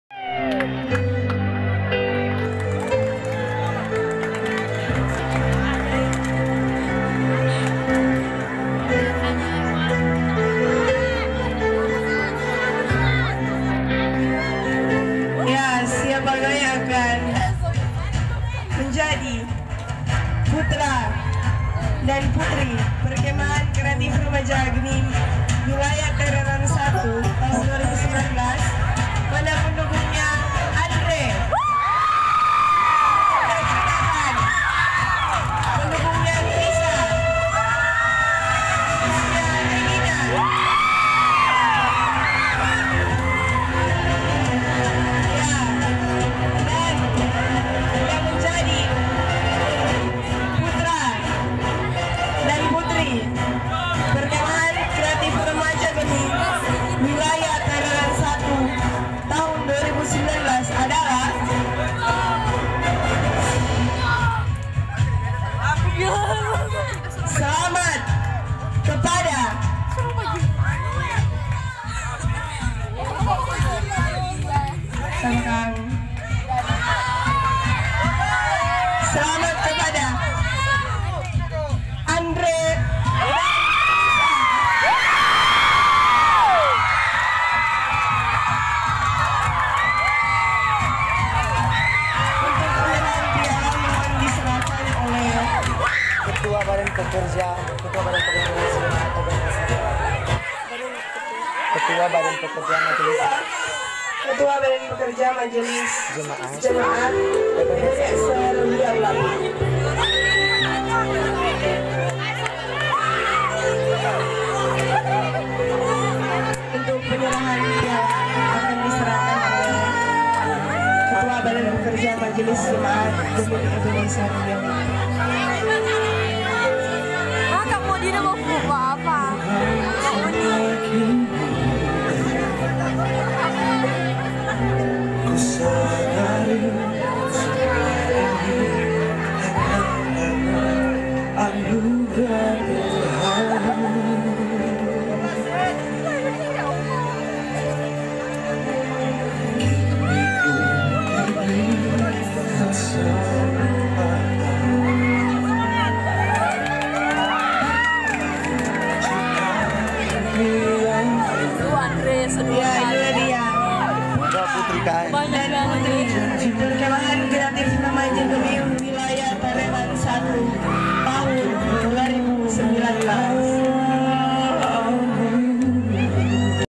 Pemilihan Putra Putri PKRW Tareran Satu 2019
Putra PKRW : Andre Rompas (GMIM Imanuel Wiau Lapi)
Putri PKRW : Chrisa Rumangu (GMIM Taar Era Rumoong Atas)
Putra Favorit : Jonathan Lumintang (GMIM Imanuel Rumoong Atas 2)
Putri Favorit : Regina Worotitjan (GMIM Eben Haezer Wiau Lapi)

@Lapangan Desa Wiau Lapi Barat
*GMIM Imanuel Wiau Lapi
*GMIM Eben Haezer Wiau Lapi